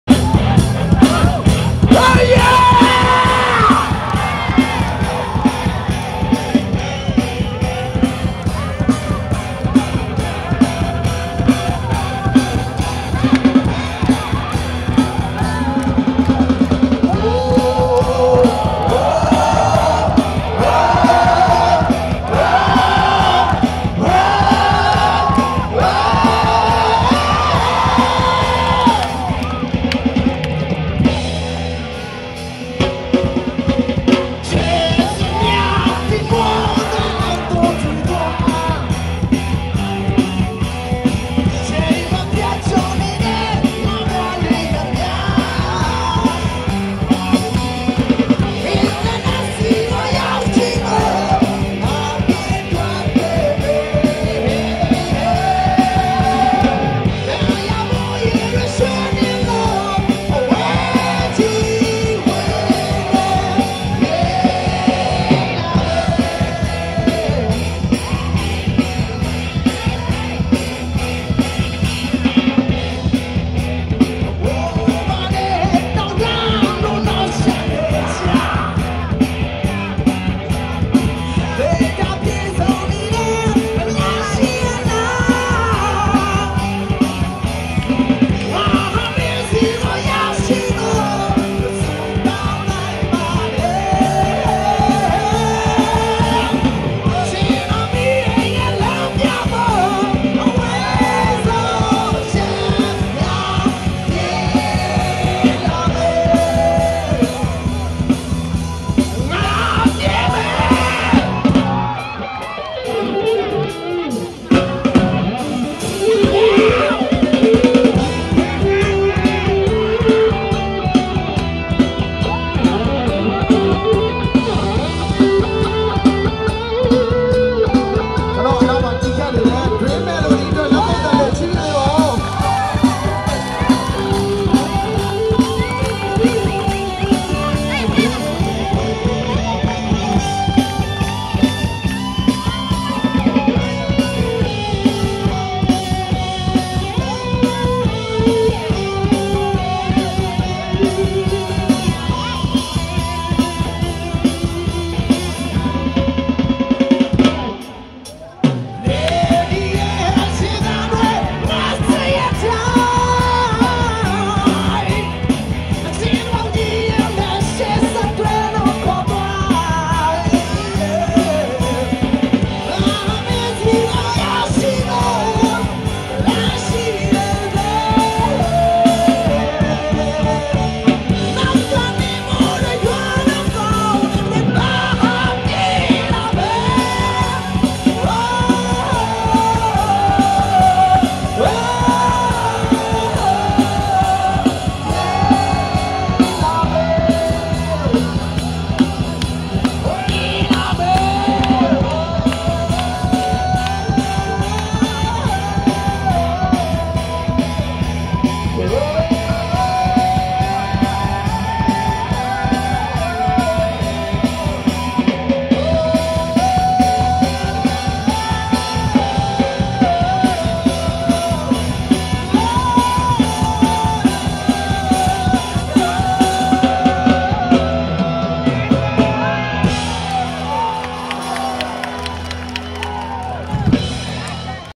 Rock&Roll thailand ! Myo gyi / Mae Sot
Welcome in Thaïland with burmese crazy young people ;)
Love Thaïland ! Love Myanmar !